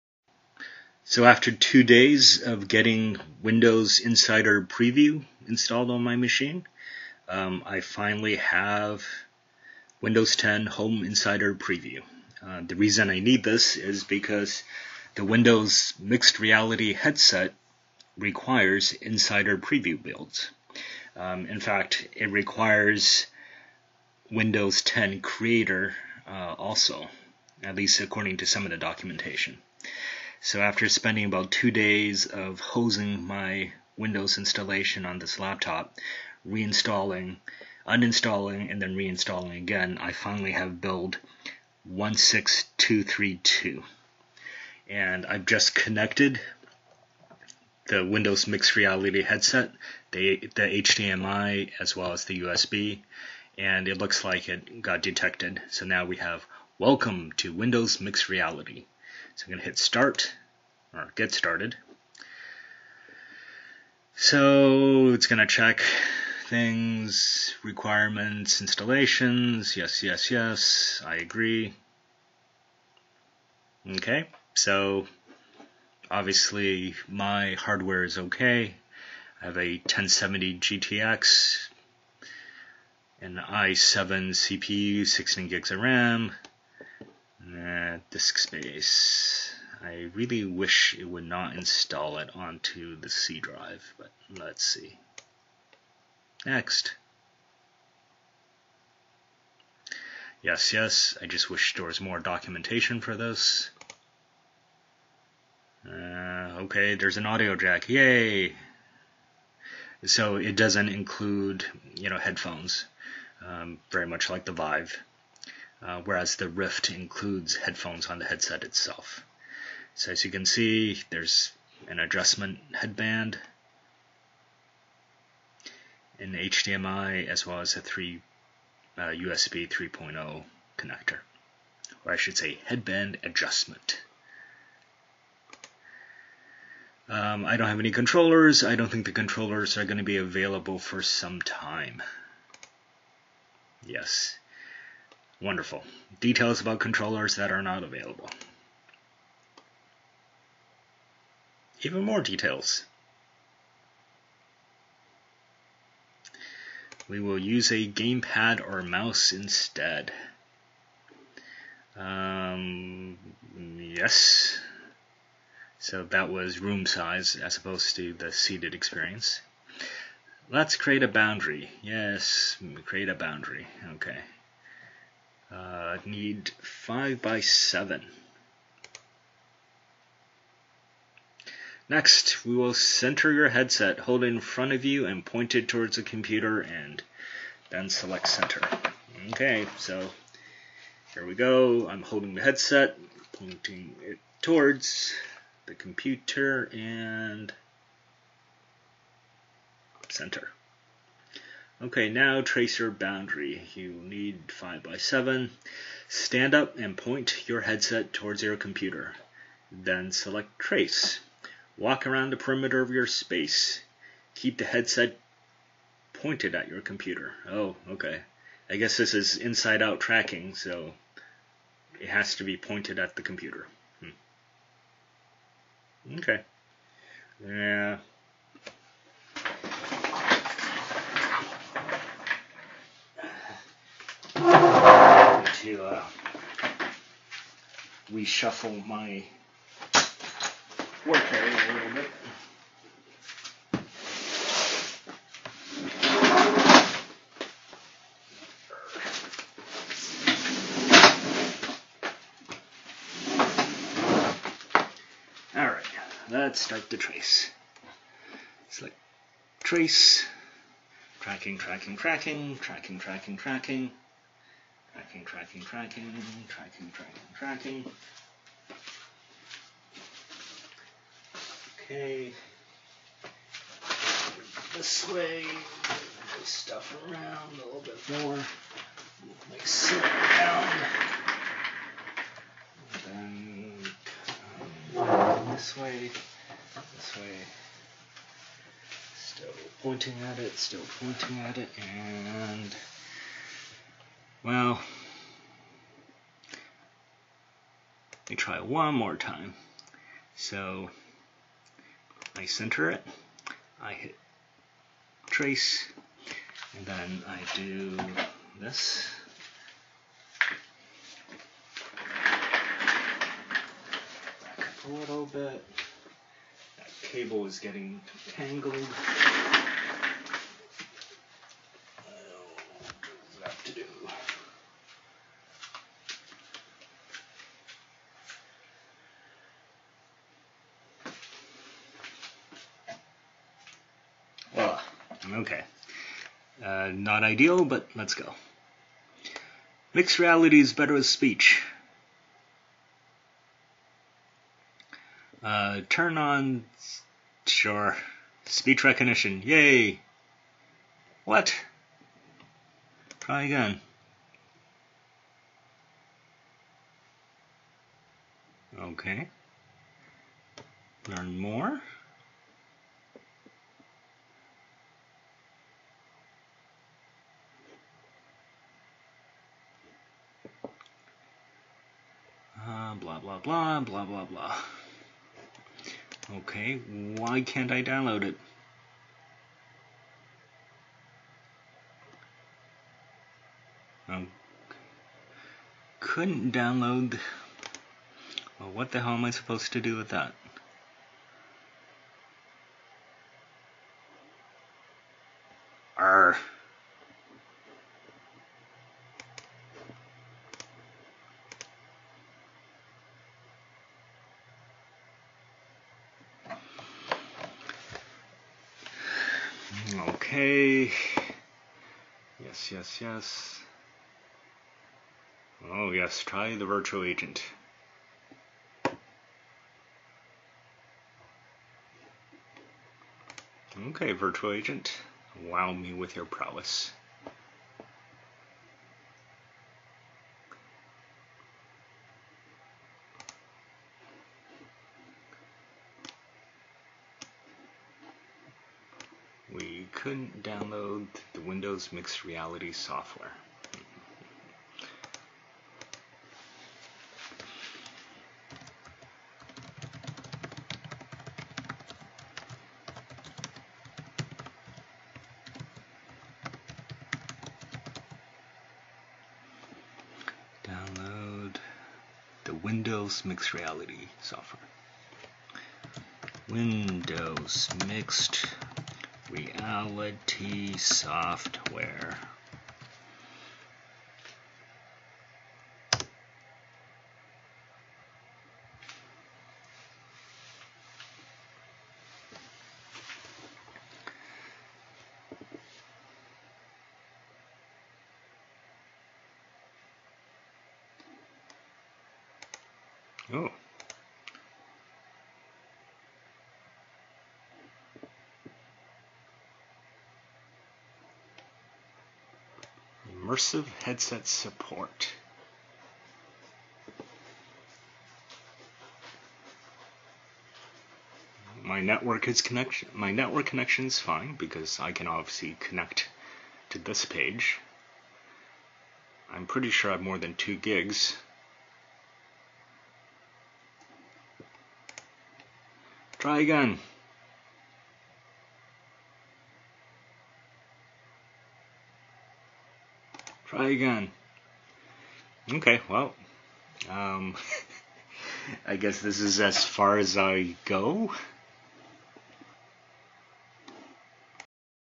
Windows Mixed Reality Headset Setup (Problem)
After spending two days messing around with Windows 10 Insider Preview builds and crippling my laptop, the moment of reckoning is at hand! Well, the setup got stuck at "We couldn't download the Windows Mixed Reality Software". I'm on Windows Insider build 16232. Help? This thing is giving me an aneurysm. Who is in charge of product management for this thing? Is it too much to ask to have a detailed step by step setup instructions? Not doing a good job at capturing heart and minds of developers here. I'm going to give it a couple of more days but I'm starting to think that I should just return this.